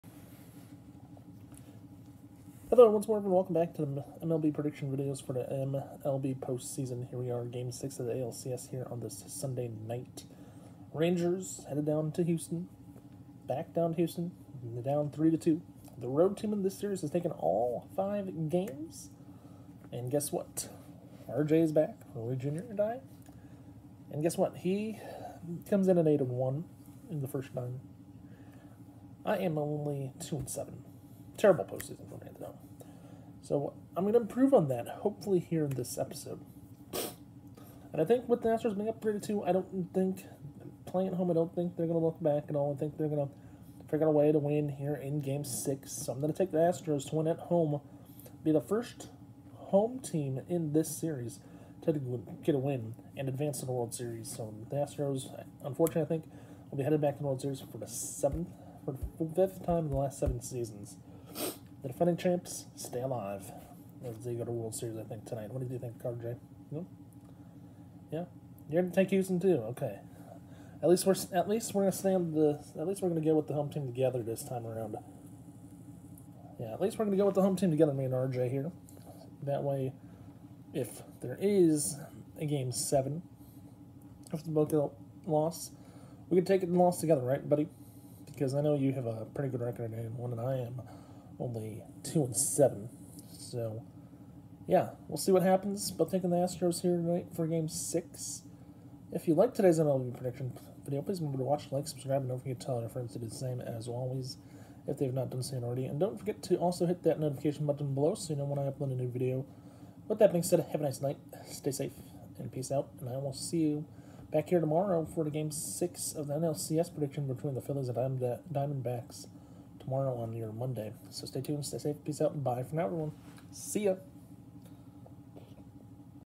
2023 MLB Postseason Predictions #7
Here is todays @MLB prediction for game six of the ALCS between the @Rangers and @astros. Coming into tonight RJ is 8-1 while I sit at 2-7. Tonight though me and RJ think @astros closes out the series at home. I hope everyone enjoys the video.